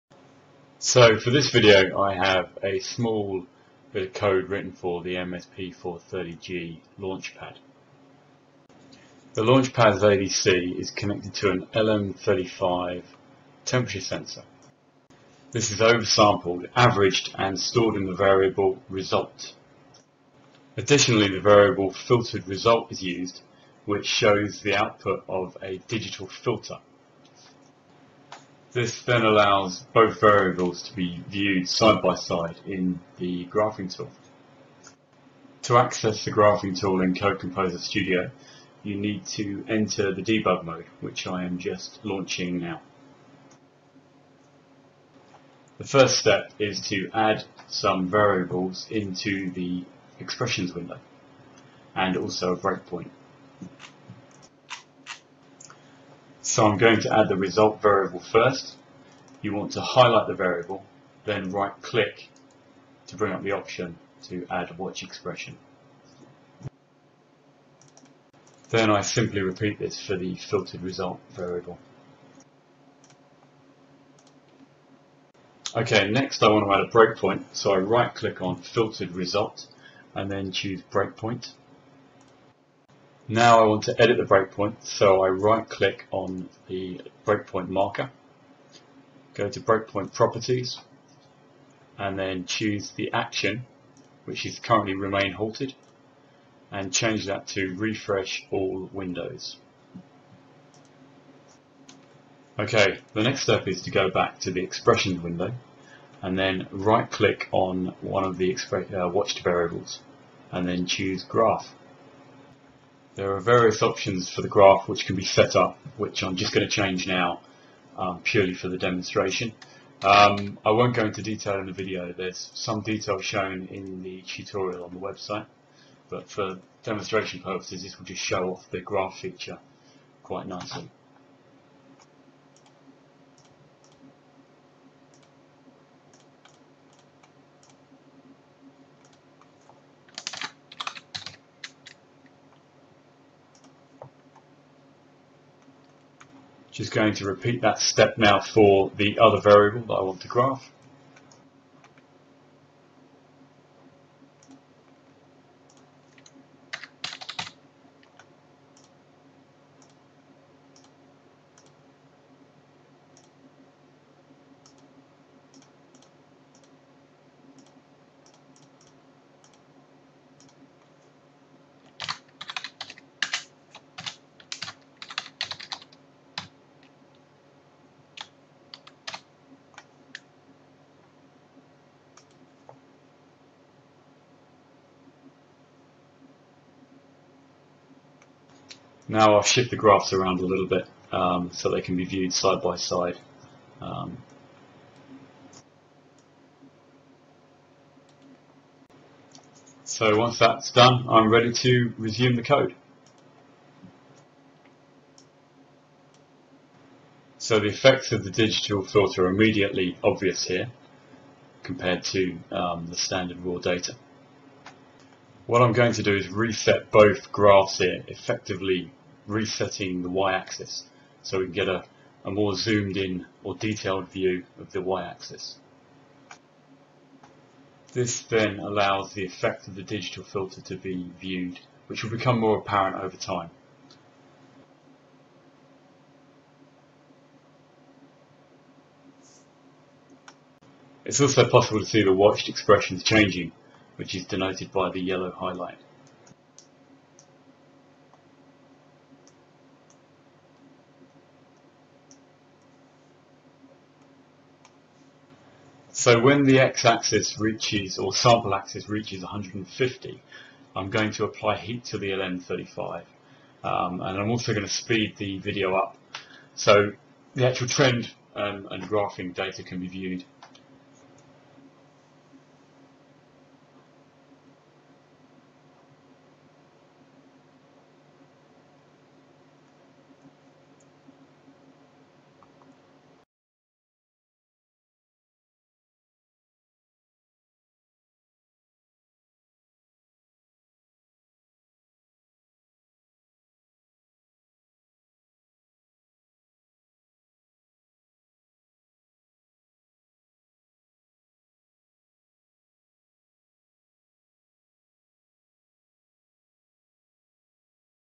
Code Composer Studio Graphing Tool Tutorial with MSP430G Launchpad
In this Code Composer Studio graphing tool tutorial, graphing variables will be demonstrated in Texas Instruments IDE.  The process is fairly straightforward and is shown in this video as well a full tutorial on my website at the following location

If you find this useful don't forget to give it the thumbs up :-)